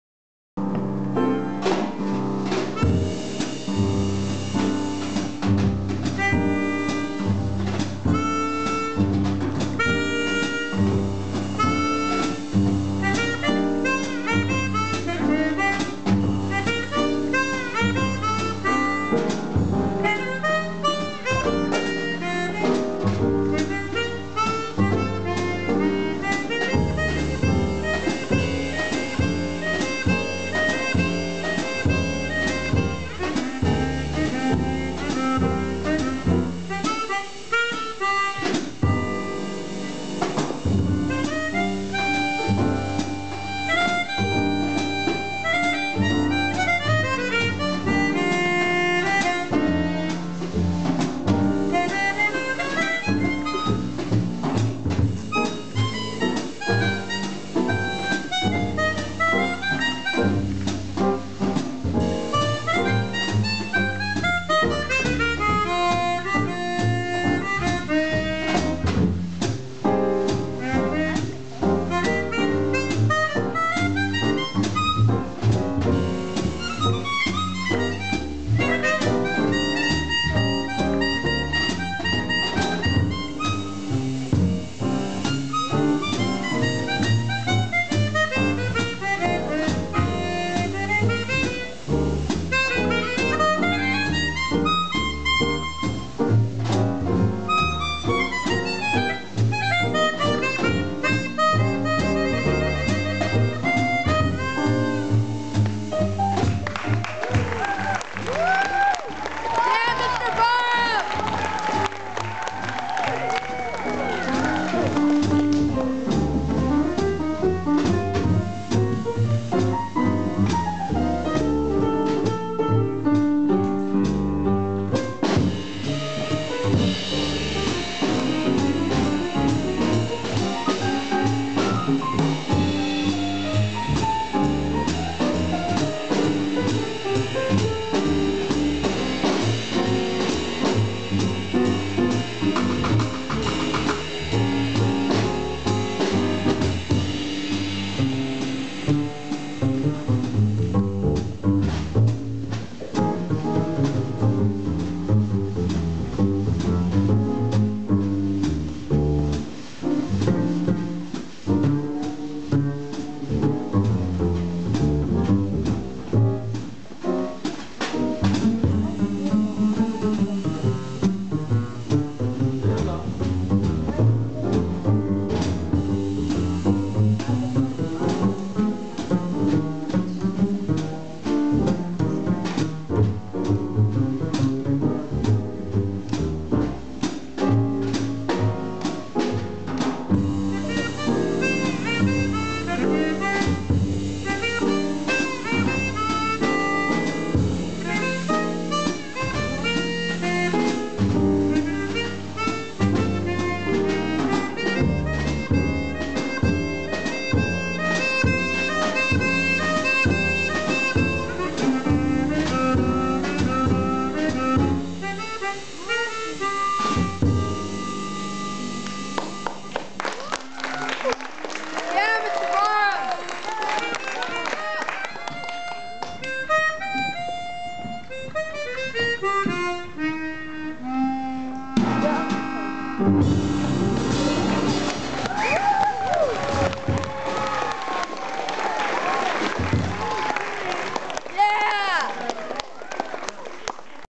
EC Coleman and the EC3 Trio with Frank Barham
EC3 Informance at the Howard School for Jazz Appreciation Month, 4/28/11